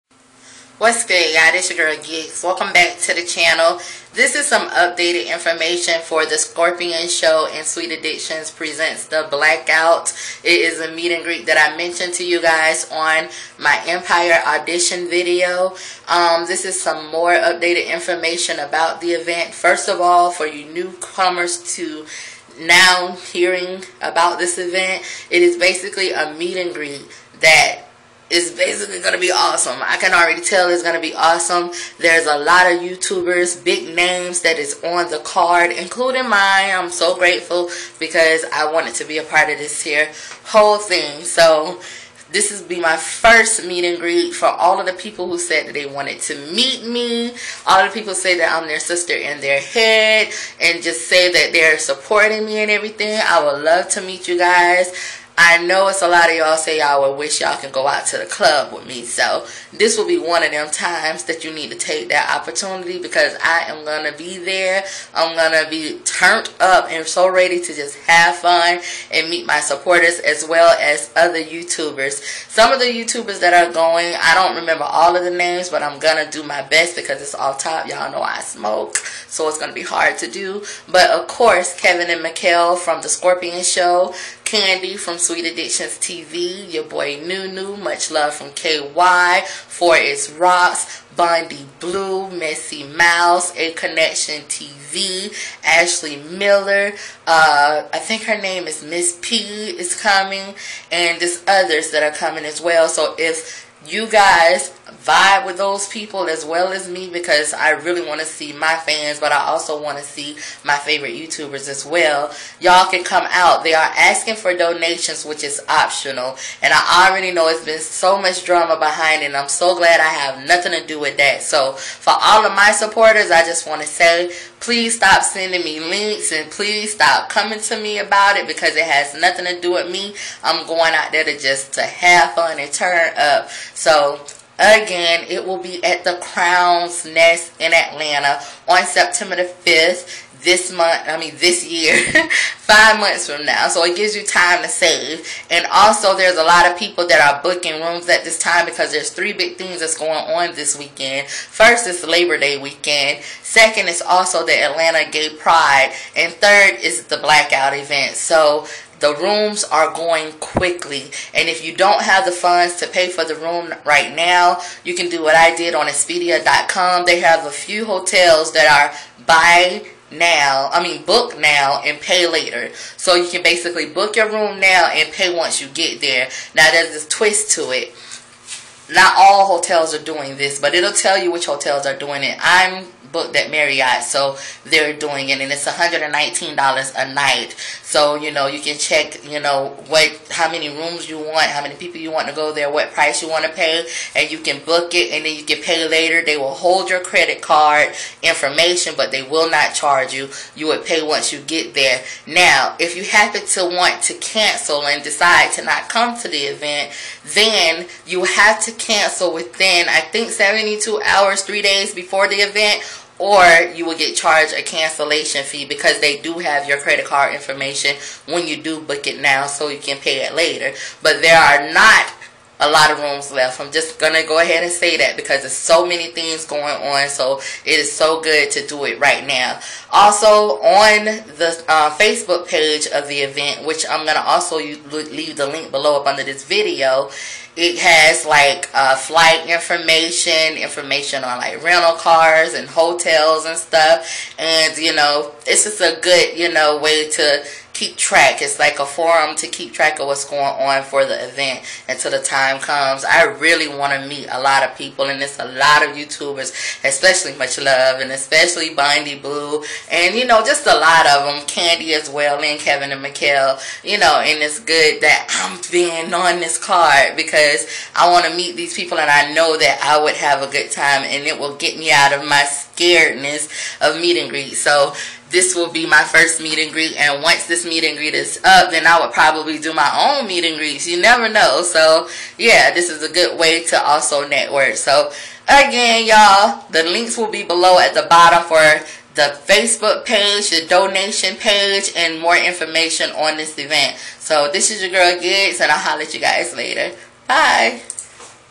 UPDATE : The Skorpion Show & SweetAddictionsTv Blackout Info..
The Skorpion Show & SweetAddictions TV Presents The Blackout. The event will also be attended by popular Youtube bloggers MessyMyles, AshleyMiller, MissTP90, MuchlovefromKy, 4ItsRox, AconnectionTV, BondyBlue, Jowork,YoBoiNewnue, Giggs and more! Its going down September 5th, 2015 during Labor Day Weekend at the Crowe's Nest in Atlanta, GA. This is your opportunity to meet all of us, so we’re hoping you have already started your travel plans to come.

All of the Youtube bloggers attending will be contributing to making this event happen. Also, because this event is free for everyone of all ages, we're hoping that you will be able to donate to this event. The money that you donate, no matter how big or small will go towards the renting of the venue, the catering services, and to the DJ.

We'll be accepting donations until one week before the event which is Saturday August 29th, 2015.